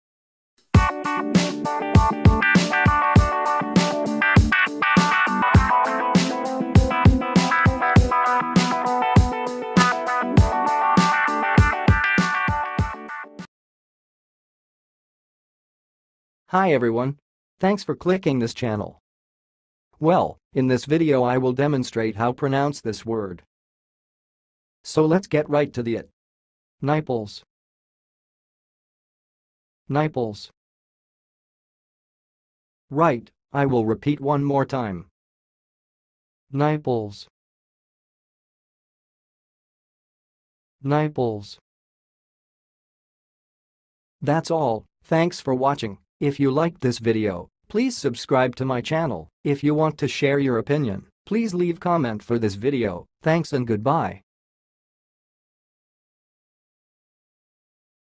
NIPLES - How to pronounce it?
NIPLES - Speak like a pro: How to say Niples in 2023

Nipples are small raised areas of tissue on the surface of the breasts, typically darker in color and more sensitive than the surrounding skin.
They are a distinguishing feature of both male and female bodies, serving various functions including breastfeeding and sexual stimulation.
Nipples vary in size, shape, and color among individuals.
It is natural for nipples to become erect when stimulated or exposed to cold temperatures.
Nipple piercings and nipple tattoos are examples of body modifications that some people choose to adorn their nipples with.

Pronunciation:
To pronounce 'nipples', say 'ni-puhls'.
Alternative variants include 'ni-puhlz' and 'nip-uhls'.

Test yourself by speaking the following examples:
1. She felt a sharp pain in her nipple when her baby started breastfeeding.
2. He got his nipples pierced as a form of self-expression.
3. The color of her nipples is darker than the surrounding skin.
4. Cold weather caused her nipples to become erect.
5. The nipple sensitivity increased her sexual pleasure